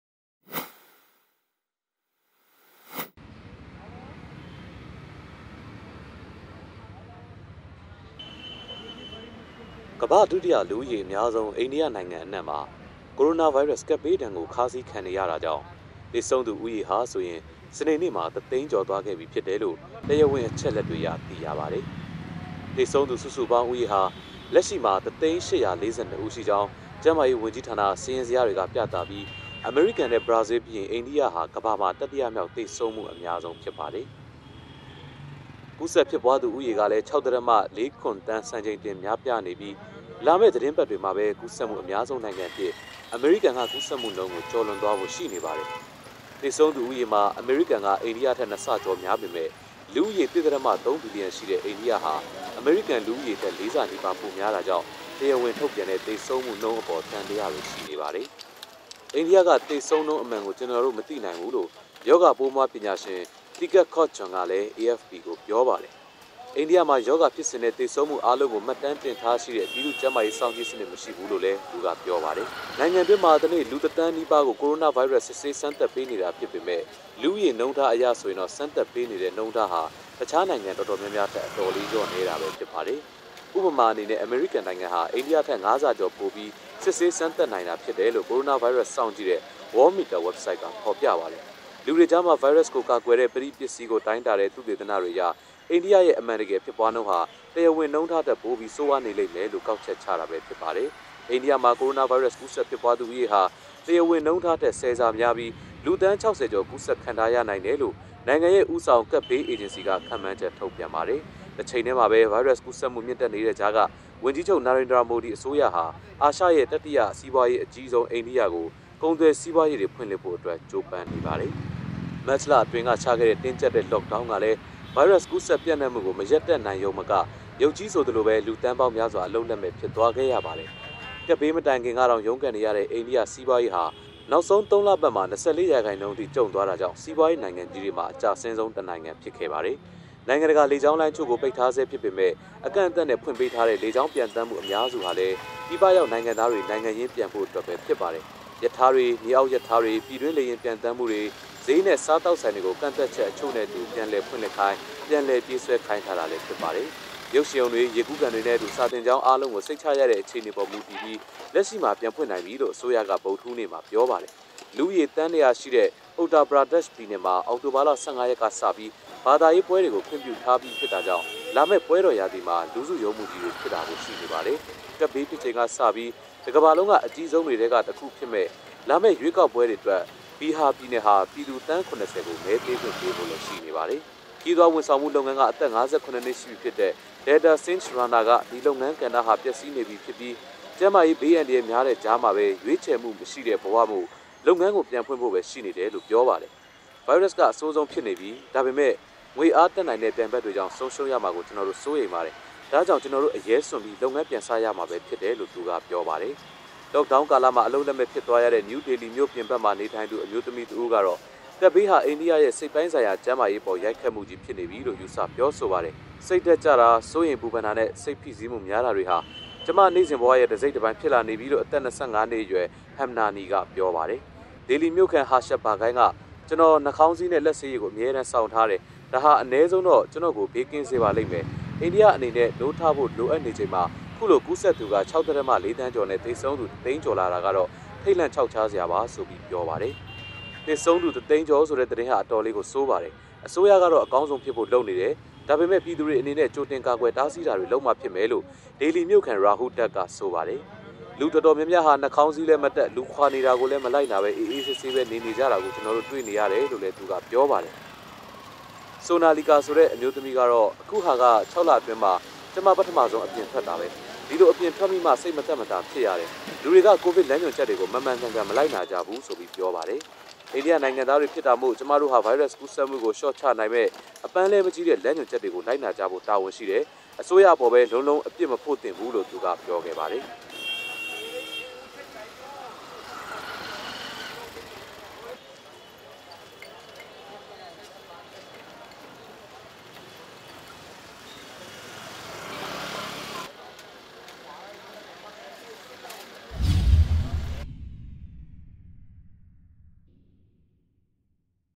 အိန္ဒိယမှာ ကိုရိုနာဗိုင်းရပ်စ်ကြောင့် သေဆုံးသူ ၁၀၀၀၀၀ ကျော်လာ
ကမ္ဘာ့ဒုတိယလူဦးရေအများဆုံး အိန္ဒိယနိုင်ငံအနှံ့မှာ ကိုရိုနာဗိုင်းရပ်စ် ကပ်ဘေး ဒဏ်ကို ခါးစည်းခံနေရတာကြောင့် သေဆုံးသူဦးရေဟာဆိုရင် စနေနေ့မှာ တစ်သိန်းကျော်သွားခဲ့ ပြီဖြစ်တယ်လို့တရားဝင်အချက်အလက်တွေအရ သိရပါတယ်။

တည်းဖြတ် - ဇော်မင်းထွေး

တင်ဆက် - ဆက်ဝေယံနိုင်